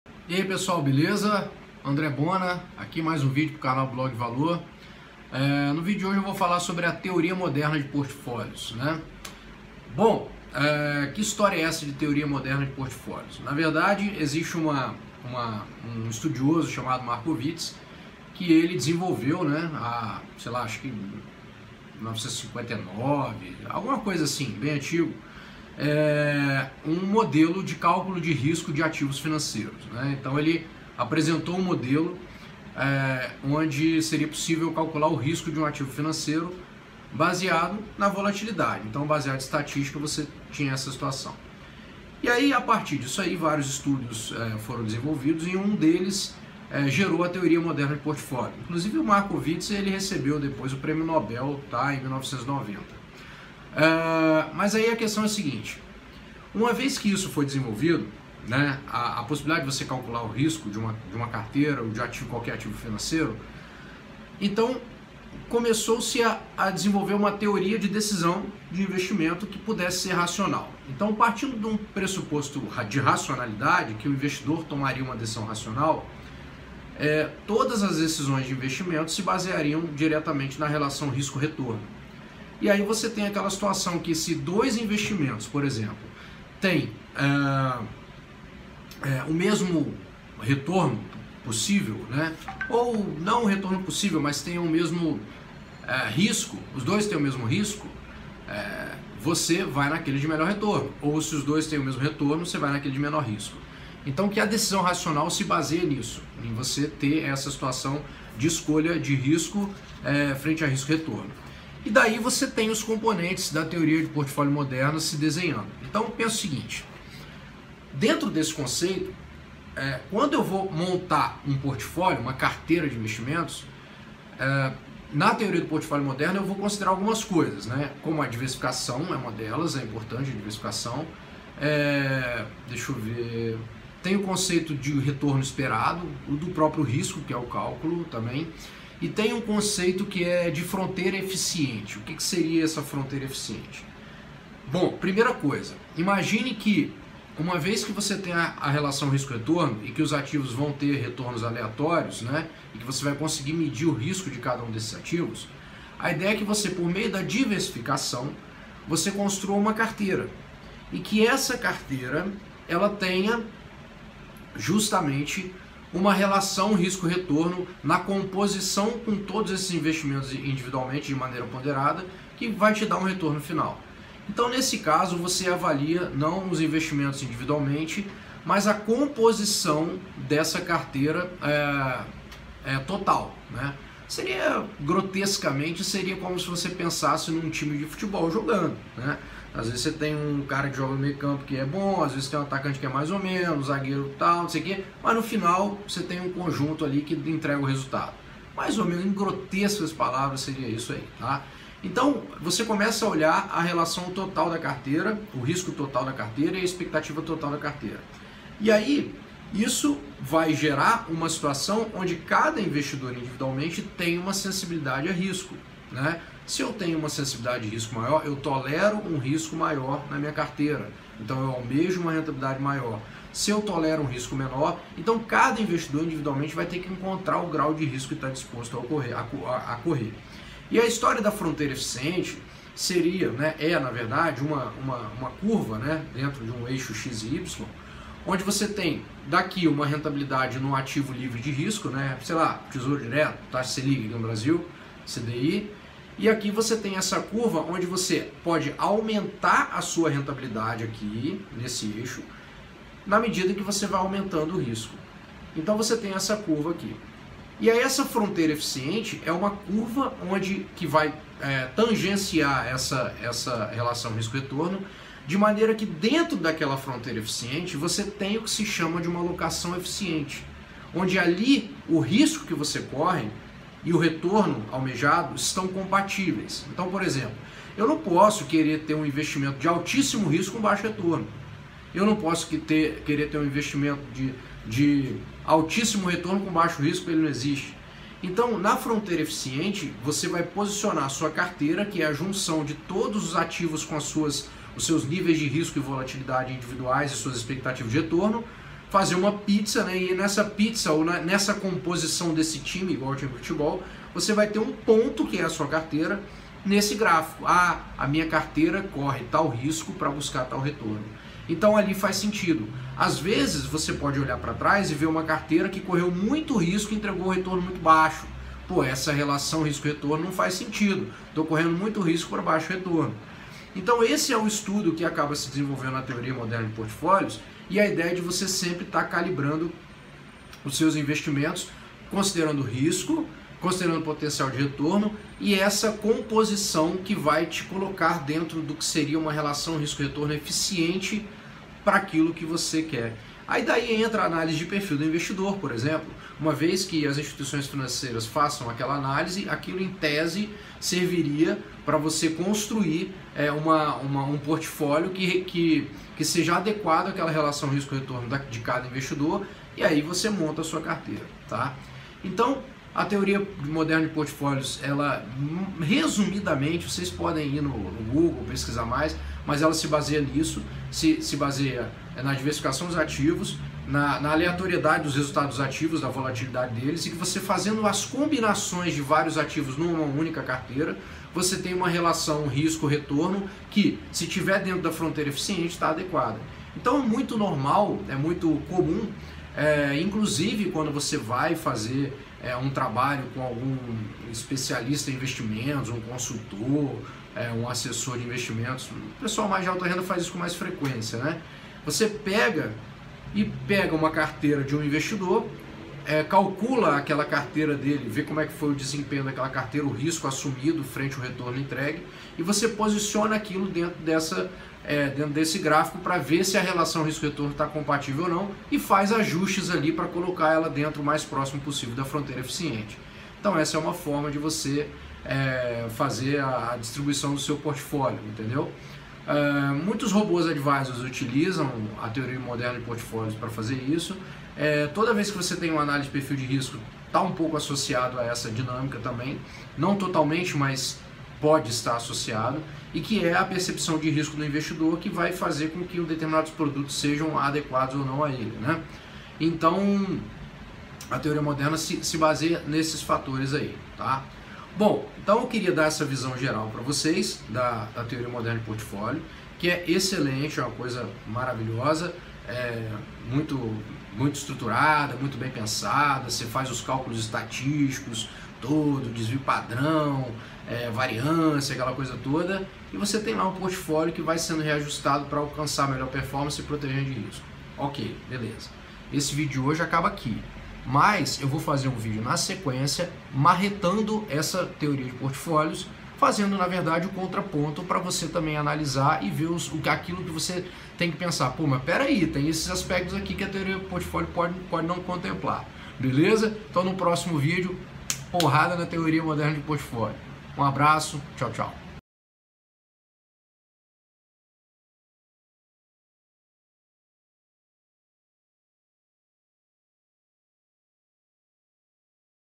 A teoria moderna de portfólio
« CONTEÚDO »

A teoria moderna de portfólio foi criada por Harry Max Markowitz.

Modelo de definição de portfólios, a partir da análise de grandes quantidades de dados, dos riscos e correlação entre ativos, cujas bases foram publicadas por Markowitz pela primeira vez em 1959.

« CONVITE »